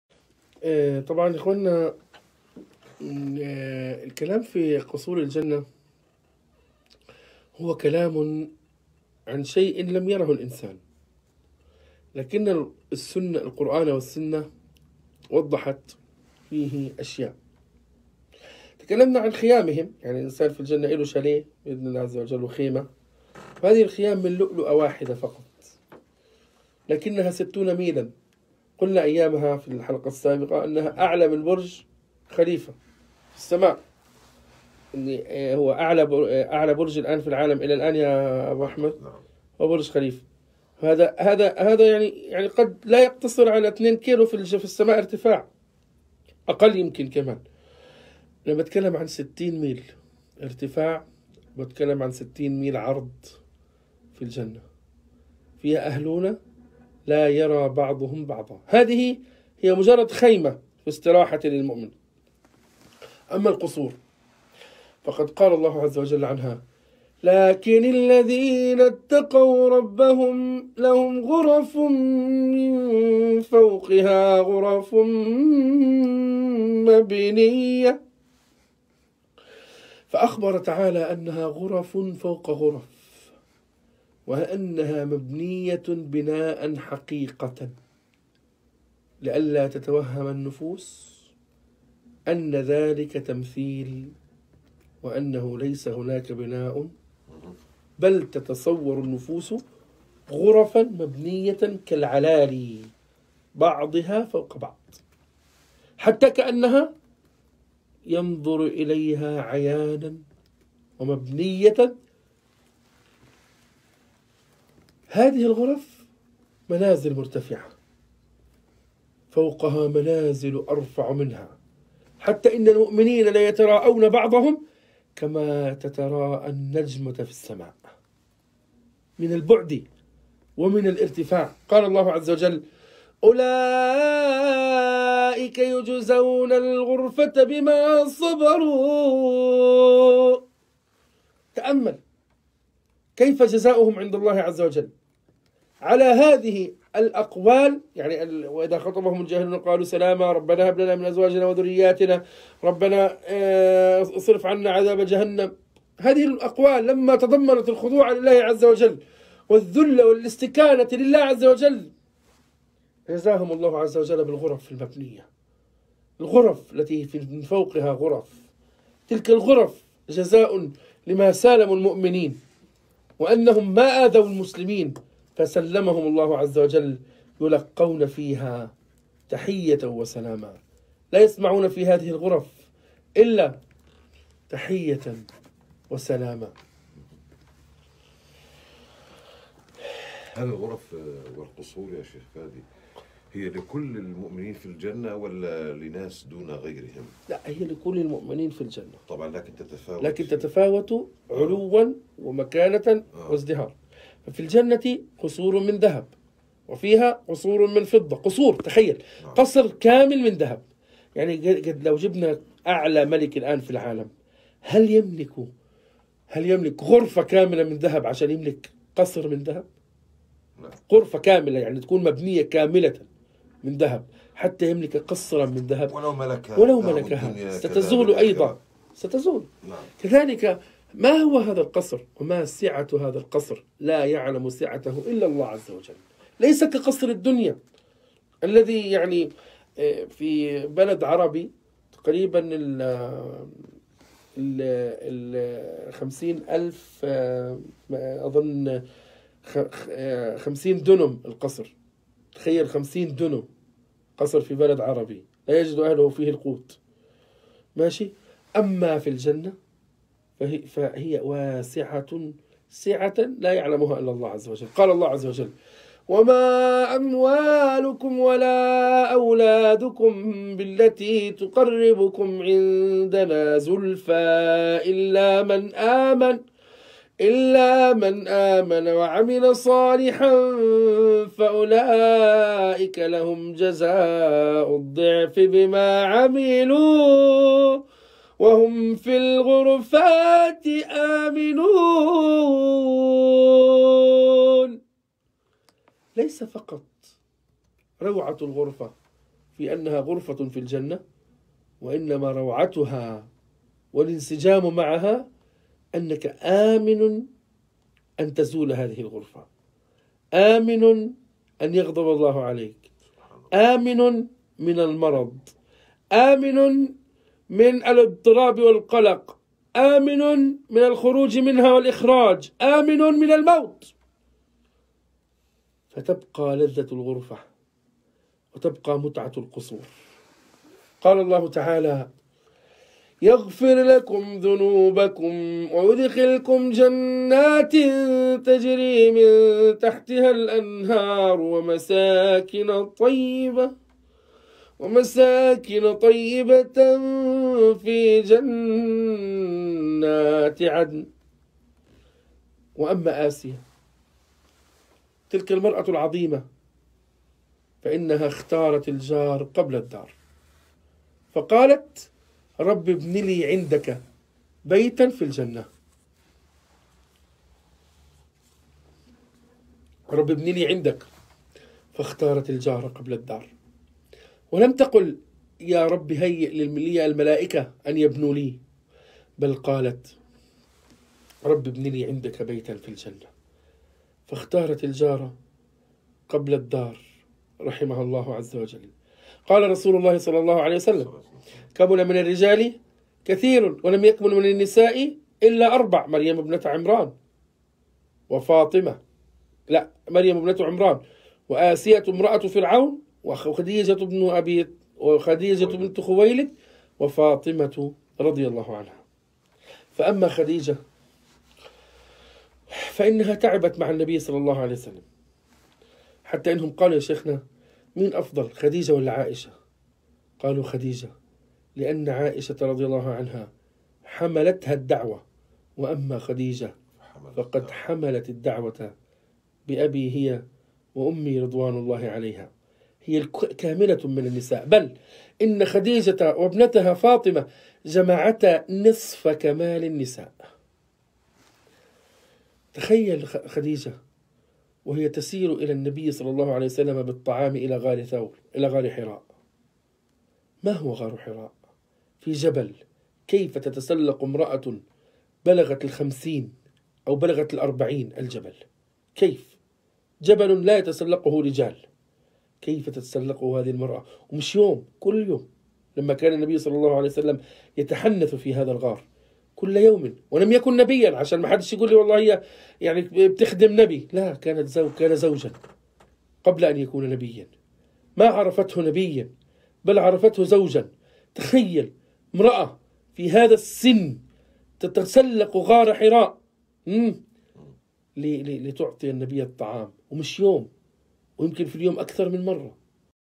الجزء الرابع : رحلة إلي الجنة غرد معنا بهذه الكلمات الموثرات إلي الجنة مع الشيخ الحبيب فادي الدالي.
صلوا على الحبيب المصطفى محمد تفلحوا 🌱
اللهم اجعل هذا العمل والنشر صدقة جارية بعدا موتي اللهم امين